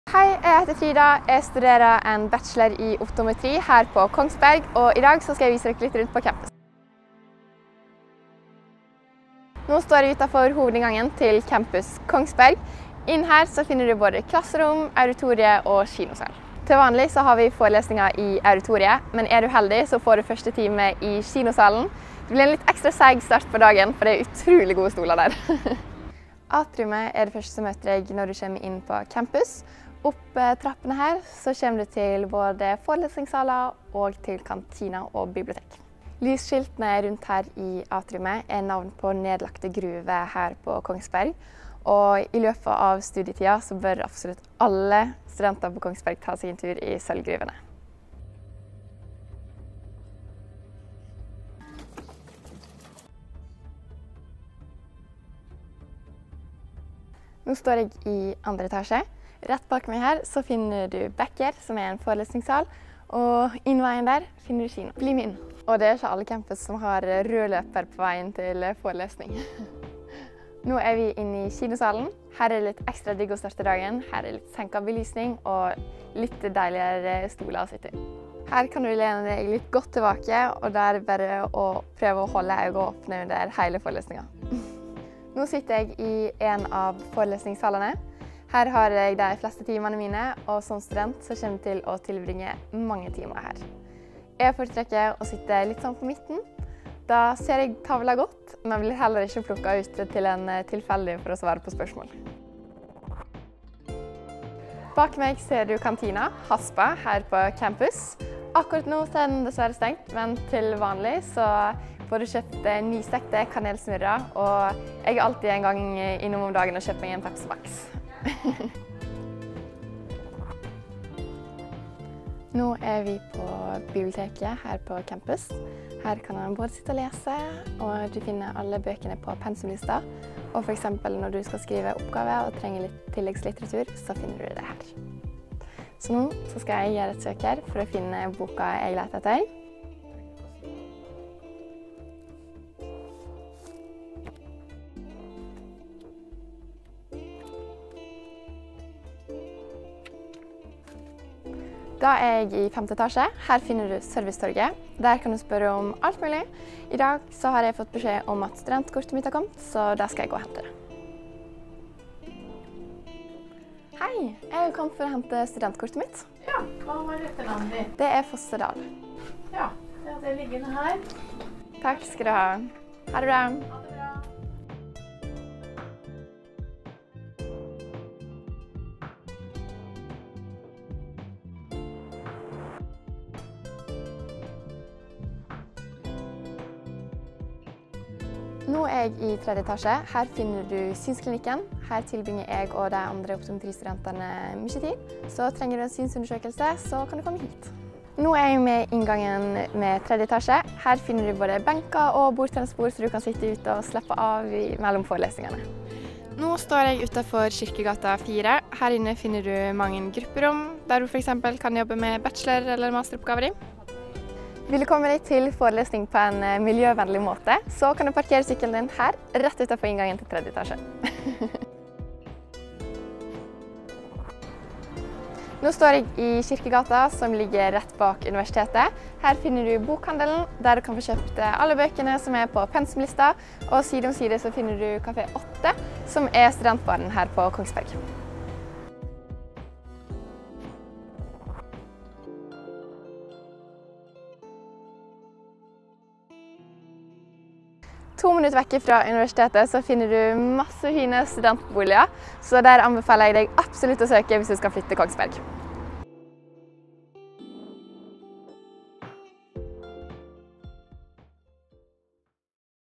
Omvisning på campus Kongsberg
Optometristudent Frida Fossedahl viser deg rundt på campus Kongsberg.

Universitetet i Sørøst-Norge - Kunnskap for fremtiden.